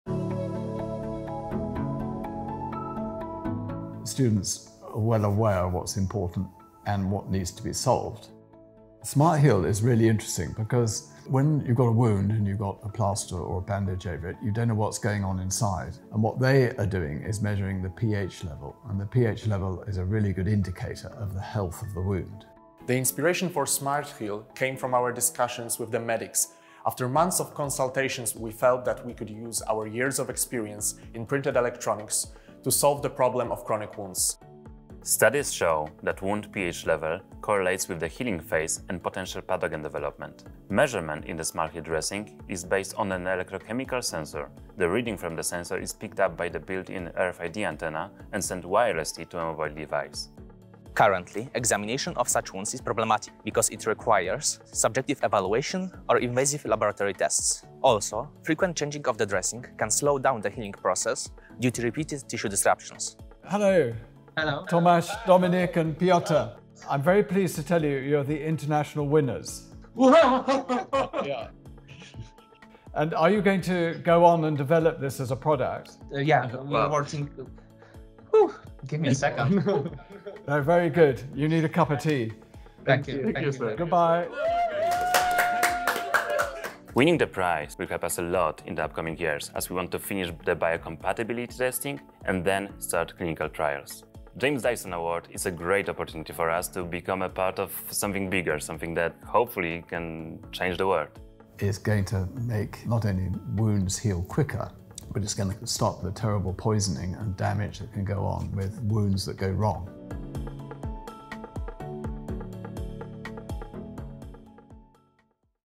The James Dyson Award 2022 international winner has been announced!
SmartHEAL is an affordable and scalable wound dressing, integrated with a smart pH sensor, which monitors the conditions of chronic wounds.